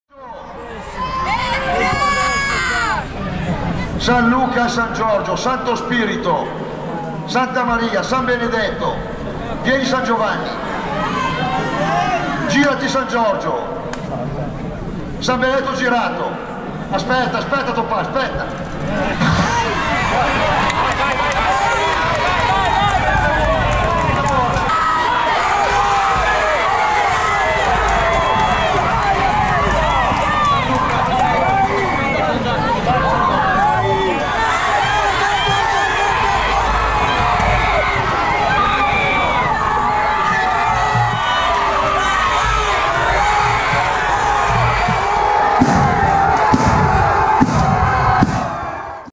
Palio, il trionfo di San Luca
C'è voluta un'ora e mezza per la mossa ma dopo sei false partenze San Luca conquista il Palio più atteso e ambito, quello della corsa dei cavalli, grazie a uno scatto decisivo di Walter Pusceddu detto “Bighino” che in sella a Calliope da Clodia porta al trionfo la contrada rossoverde. Rivivi le emozioni dell'evento con il video di Alessandro Castaldi